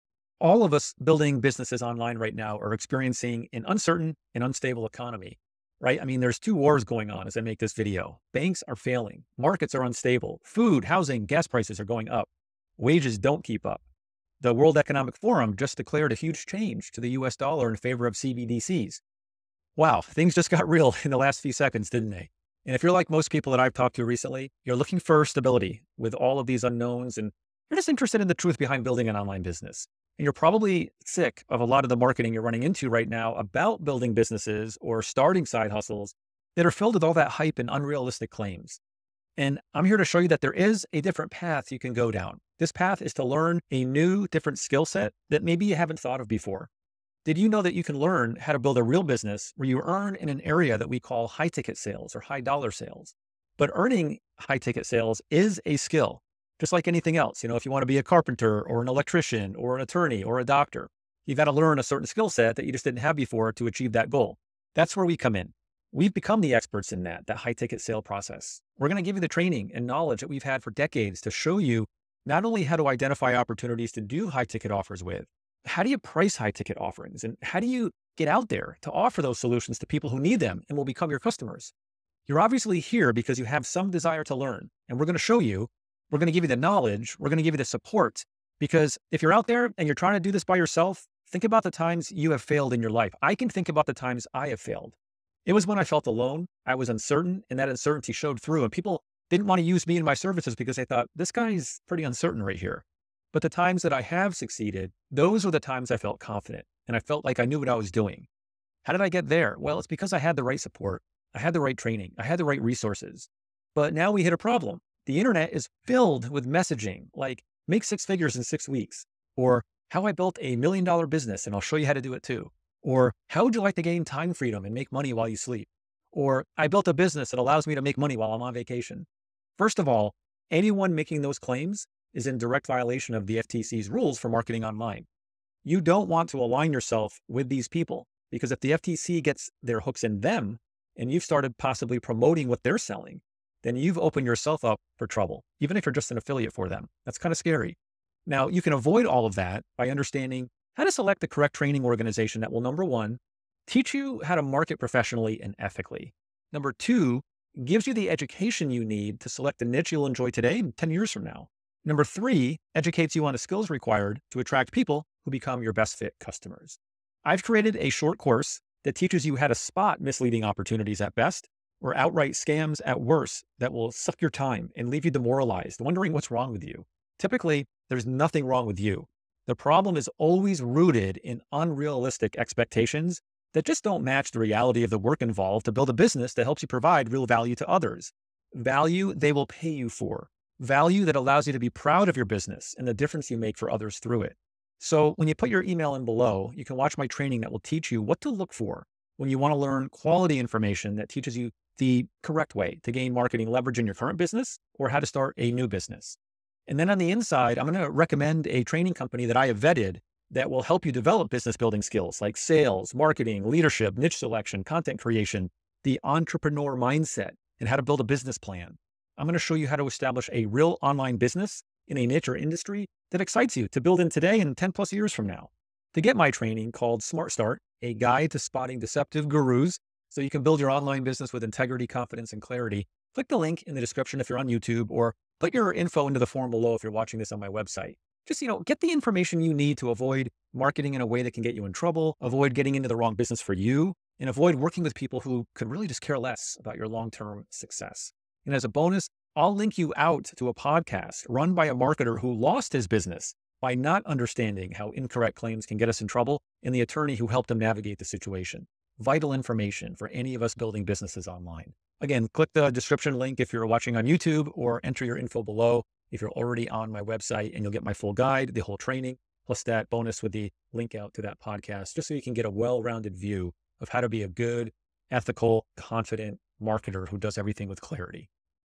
Smart Start! Spot Deceptive 'Gurus' & Build Your Online Business w/ Integrity, Confidence & Clarity
We’re all experiencing an uncertain and unstable economy. There’s two wars going on as I make this video. Banks are failing. Markets are unstable. Food, housing & gas prices are up. Wages don’t keep up. The World Economic Forum just declared a huge change to the US dollar in favor of CBDCs.

Wow! Things just got real in the last few seconds didn’t they?

If you’re like most people I’ve talked to recently, you’re looking for stability with all these unknowns, and are interested in the truth behind building an online business.

And you’re probably sick of a lot of the marketing you’re running into about building businesses or starting side hustles that are filled with hype and unrealistic claims.

I’m here to show you that there’s a different path.

This path is to learn a new, different skill set that you maybe haven’t thought of before.

Did you know that you can learn how to build a real business where you earn in an area we call “high ticket or high dollar sales”?

But earning high ticket sales is a skill. Just like anything else, whether you want to be a carpenter, electrician, an attorney or doctor, you have to learn a certain skill set that you want to achieve, but you don’t currently have.

That’s where we come in.

We’ve become the experts in that.

We’re going to give you the training and knowledge that we’ve had for decades to show you not only how to identify opportunities to do high ticket offers with, but how you price high ticket offerings and how you get out there to offer those solutions to people who need them and will become your customers.

You’re obviously here because you have some desire to learn.

We’re going to show you.

We’re gonna give you the knowledge.

We’re gonna give you the support, because if you’re out there and you’re trying to do this by yourself, think about the times you’ve failed in your life.

I can think about the times I’ve failed.

It’s when I felt alone, I was uncertain and that uncertainty showed through and people didn’t want to use me and my services because they thought, “this person’s uncertain”

But the times I”ve succeeded, those were the times I felt confident, and I felt like I knew what I was doing.

Why did I get there?

It’s because I had the right support, I had the right training, and I had the right resources.

But now we hit a problem.

The Internet is filled with messaging like:

“Make six figures in six weeks” OR

“How I built a million-dollar business and I’ll show you how to do it too” OR

“How would you like to gain time freedom & make money while you sleep” OR

“I built a business that allows me to make money while I’m on vacation”

First of all, anyone making those claims is in direct violation of the FTC’s rules for marketing online.

Now, you can avoid all that by understanding how to select the correct training organization that:

One, teaches you how to market professionally and ethically
Two, gives you the education you need to select a niche you’ll enjoy today and ten years from now
Three, educates you on the skills required to attract people who become your best fit customers

I’ve created a short course that teaches you how to spot misleading opportunities that will suck your time and leave you demoralized, wondering what’s wrong with you.

Typically, there’s nothing wrong with you. The problem is always rooted in unrealistic expectations that don’t match the reality of the work involved to build a business that helps you provide real value to others.

Value they will pay you for.

Value that allows you to be proud of your business and the difference you make for others through it.

And then on the inside I’m gonna recommend a training company I’ve vetted that will help you develop business-building skills like sales, marketing, leadership, niche selection, content creation, the entrepreneur mindset, and how to build a business plan.

As a bonus, I’ll link you out to a podcast run by a marketer who lost his business by not understanding how incorrect claims can get us in trouble, and the attorney who helped him navigate the situation. Vital information for any of us building businesses online.